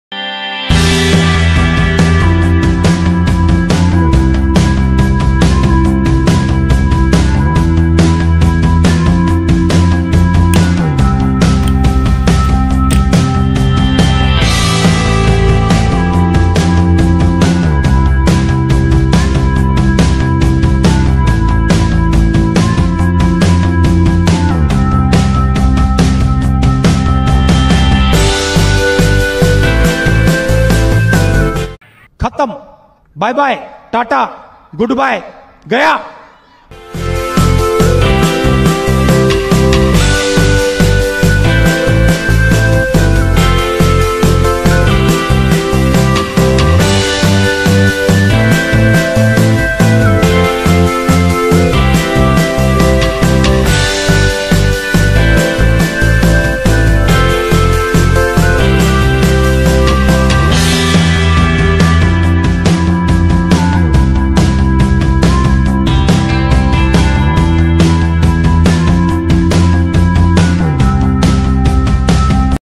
different types of crackers testing | crackers & balloon | diwali testing 2023
in this video I trying to blow balloon with crackers hope you enjoyed this amazing experiment video please like share and subscribe for more different different unique unique types of crackers videos